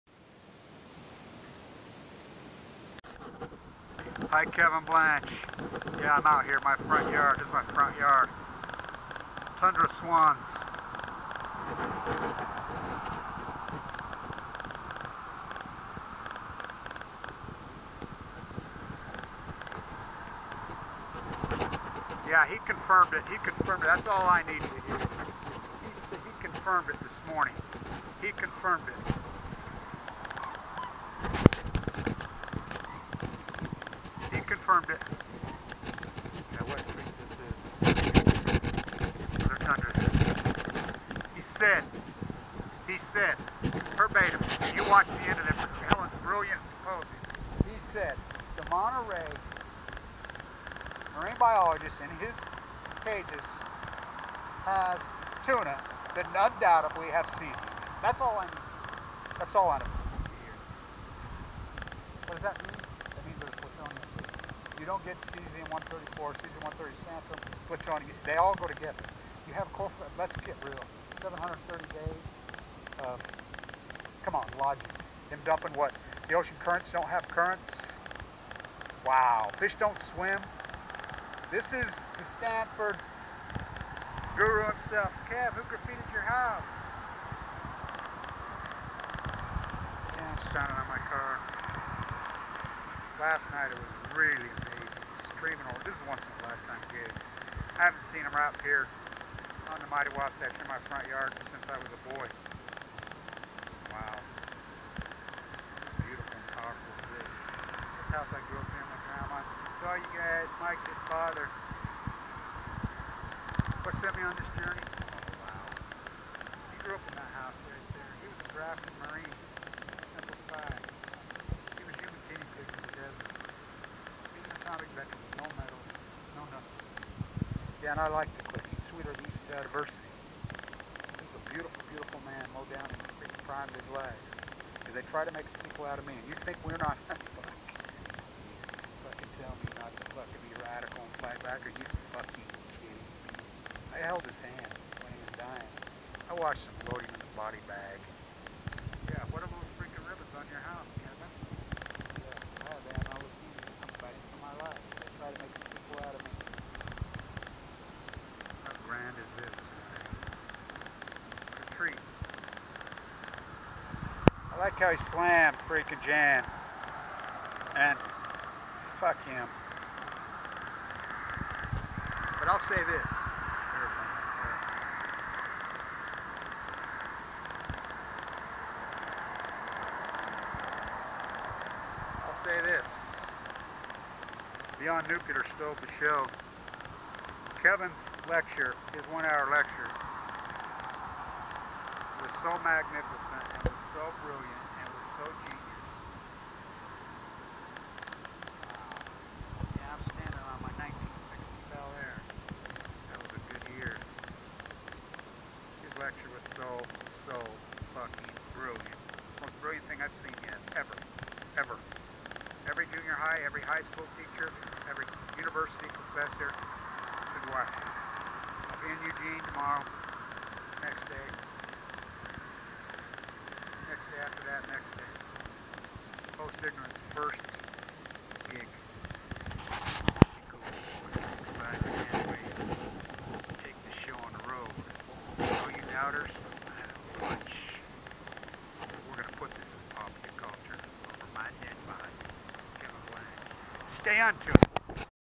Fukushiama breaking news, Nuclear fallout Confirmed in Monterey CAL. fish, keivn d blanch, take 3
3/13/13; Herbert Abrams, Stanford University School of Medicine, said yesterday; that without question there is Cesium in the monetary marine biologist's caged tuna, @

The Medical and Ecological Consequences of the Fukushima Nuclear Accident
March 12, 2013, @ the The New York Academy of Medicine symposium; that without question there is Cesium in the monetary marine biologist's cage tuna, THAT IS ALL THE confirmation I need. If there is cesium in the monetary tuna there is PU, and we are FUCKED the pacific Ocean has been officially distorted for 50,000 years by the Nuclear fucking liars,

A unique, two-day symposium at which an international panel of leading medical and biological scientists, nuclear engineers, and policy experts will make presentations on and discuss the bio-medical and ecological consequences of the Fukushima disaster, will be held at The New York Academy of Medicine on March 11-12, 2013, the second anniversary of the accident.